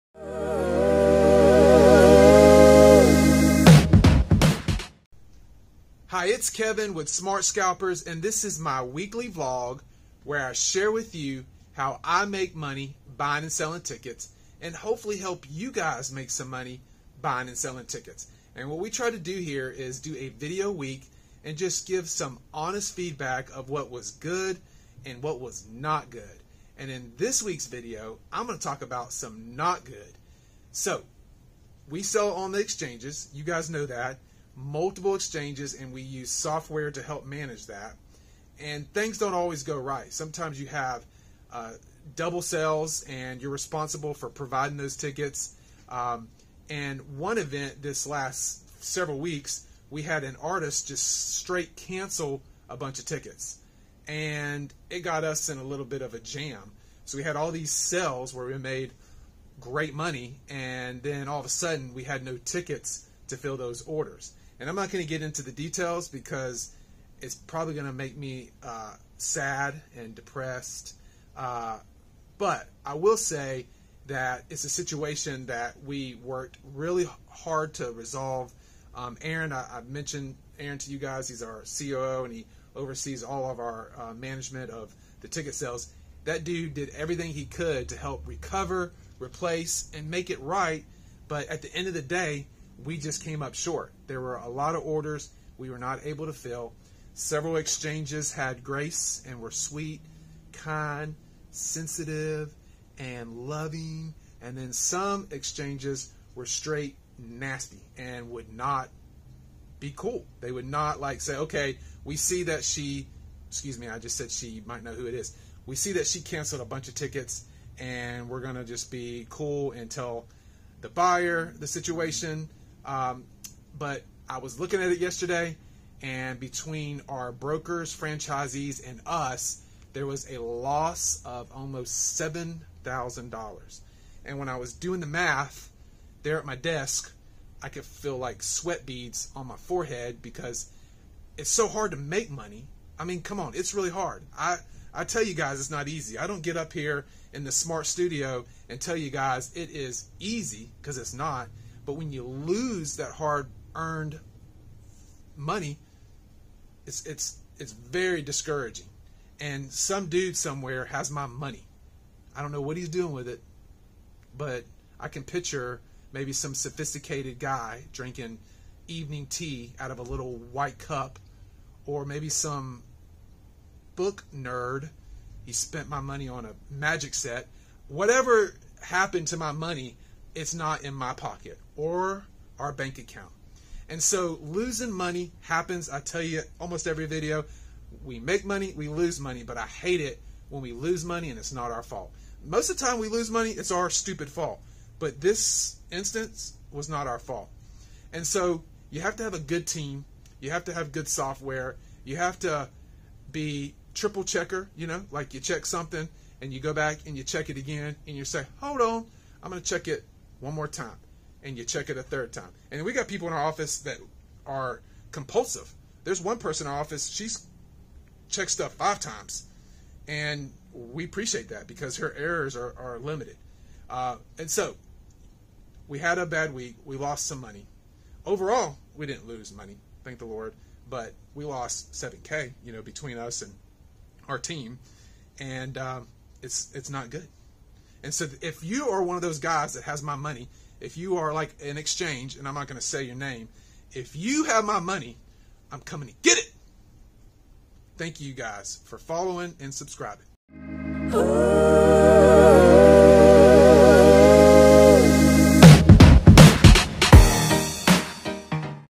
Ticket Broker Fails...Losing Money
This week's video is about losing money from events as a ticket broker. It happens as it is part of the business.